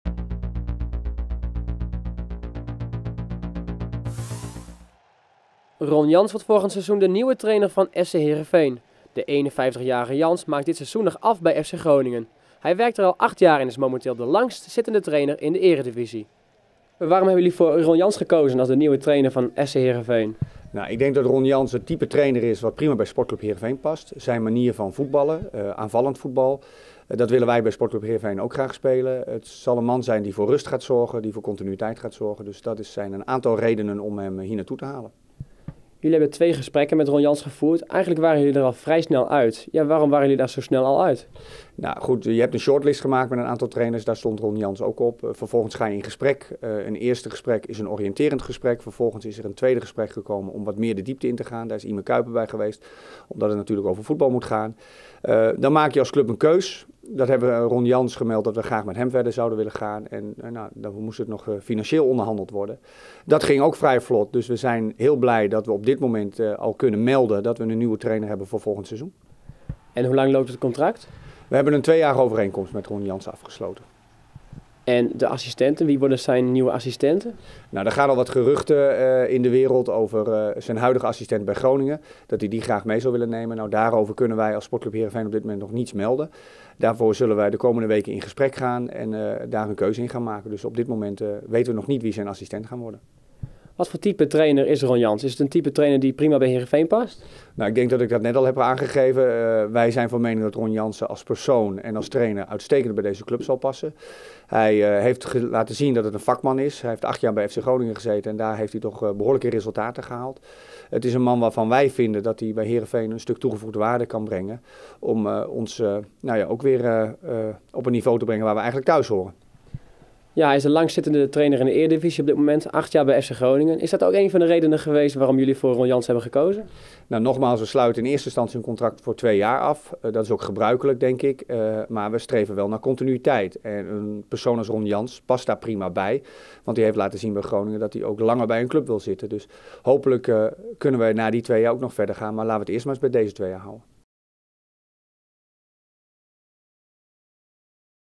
Jan van Erve: "Ron Jans past prima bij sc Heerenveen"
Commercieel directeur Jan van Erve legt uit waarom sc Heerenveen Ron Jans heeft aangesteld als de nieuwe trainer voor volgend seizoen.